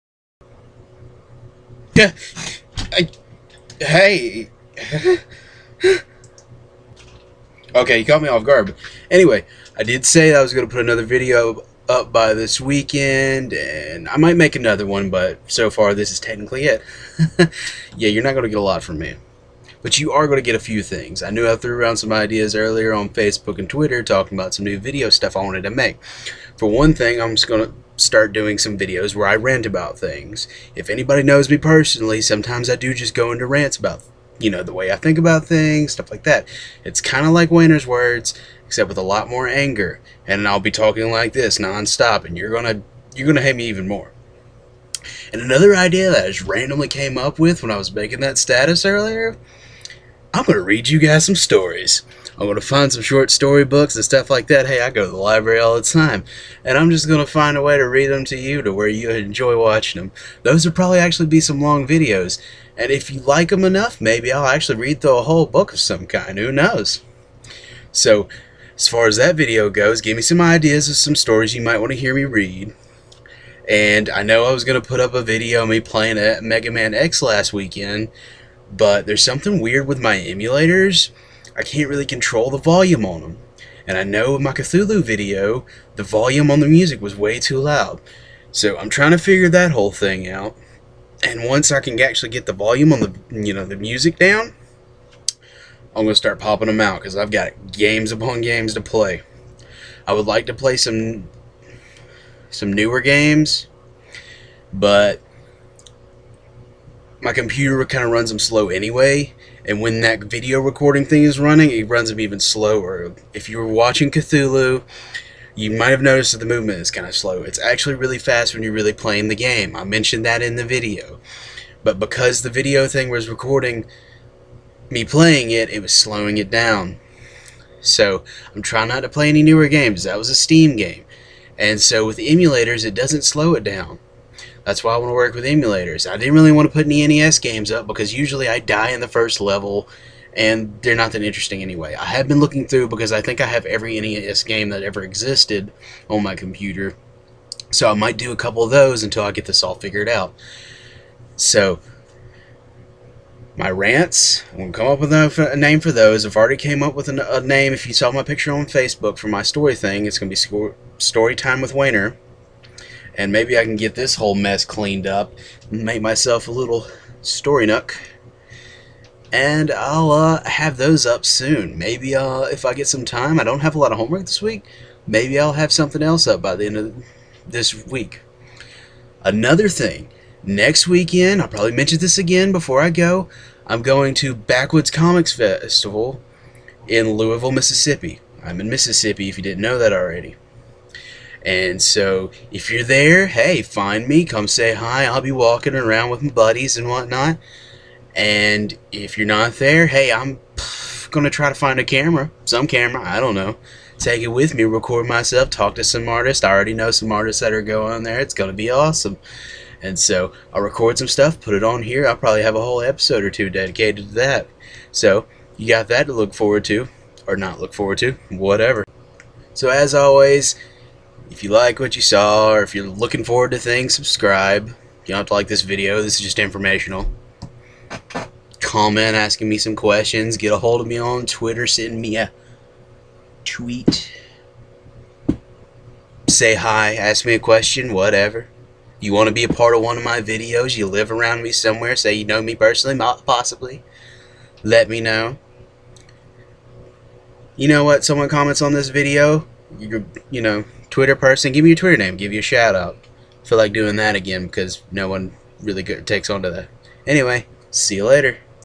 More videos to come!
I promised a video to be posted by the weekend, so here it is, I explain my problems with gameplay recording and what new shows I have in store!

And as always, you can follow Wayner on the Tweeter